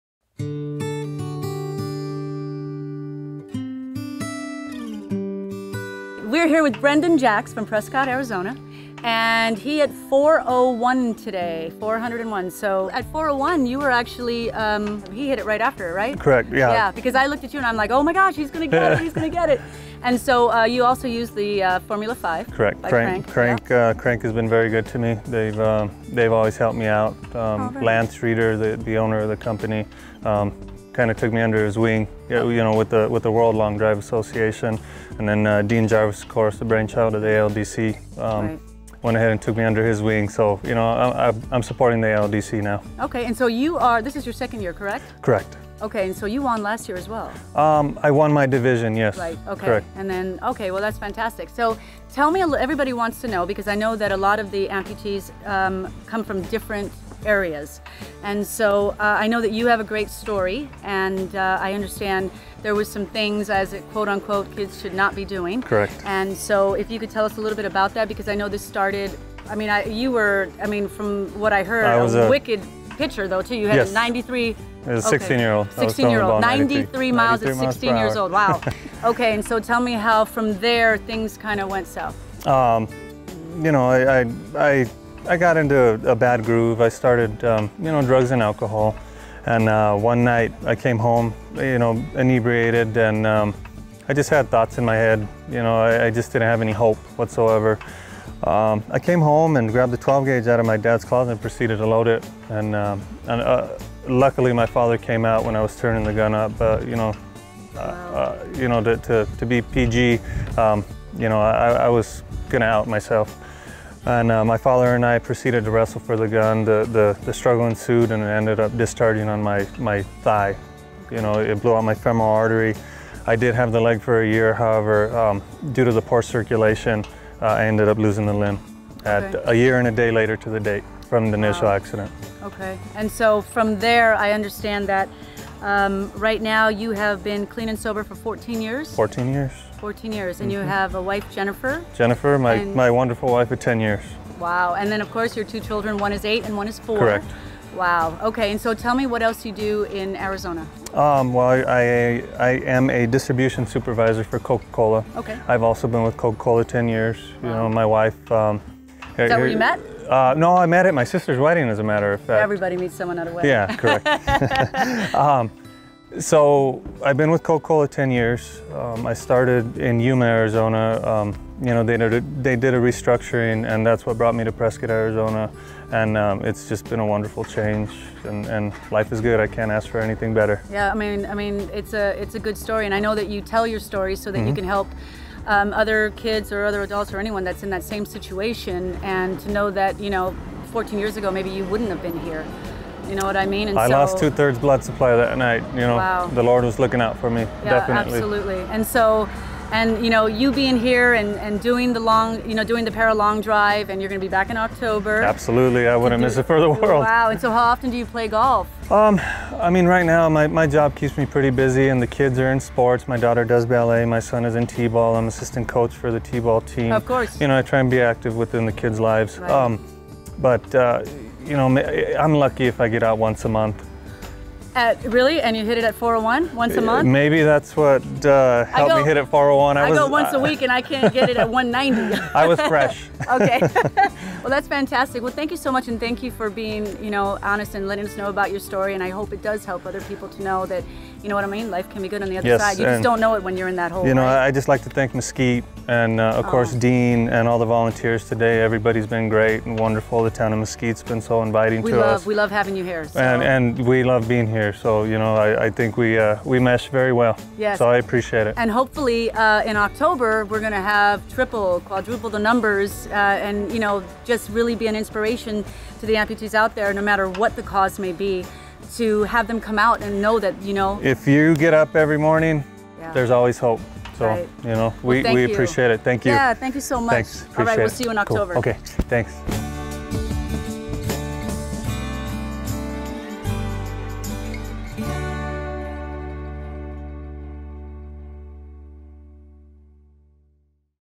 ParaLong Driver Brendon Jacks
Brendon Jacks was the first ParaLong Driver to surpass the 400-yard barrier.  He is a leg amputee golfer that has challenged ParaLong Drive Champion Jared Brentz at each competition.  Brendon tells his story after competing in the Mesquite NV ParaLong Drive Nationals on May 9, 2014.